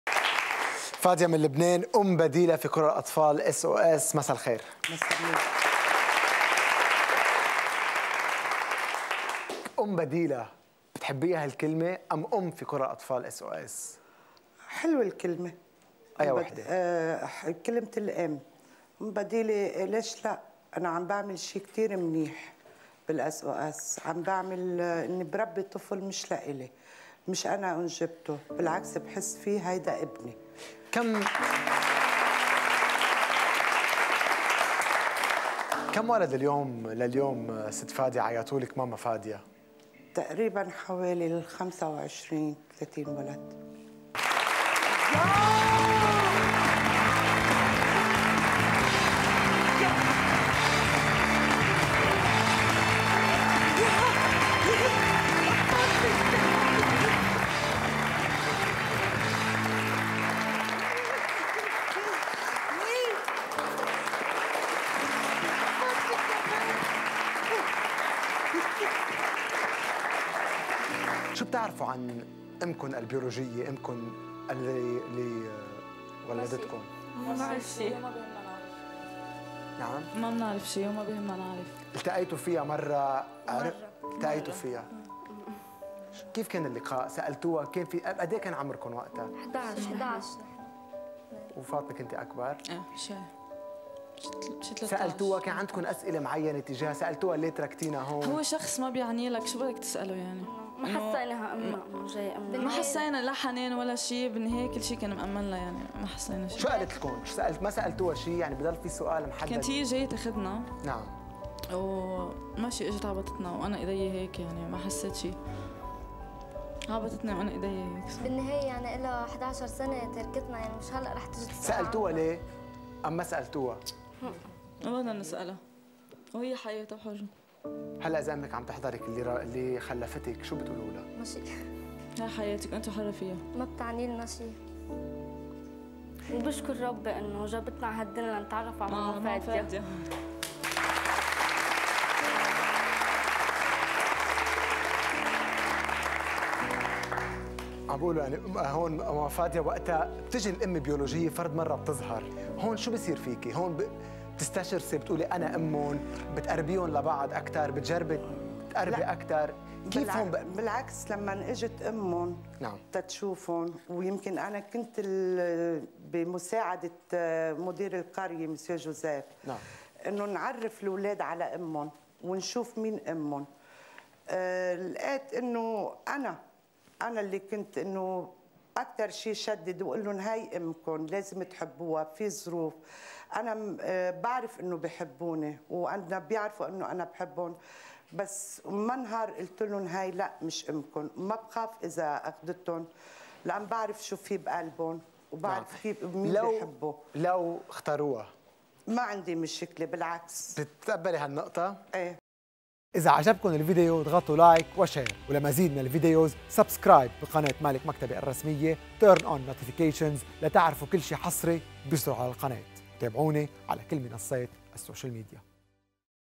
في لحظة خيار مؤثرة بين الأم التي ربت والأم التي خلّفت I فمن اختارت؟
فاديا ام كرست حياتها لتربية ٢٥ طفل في مركز SOS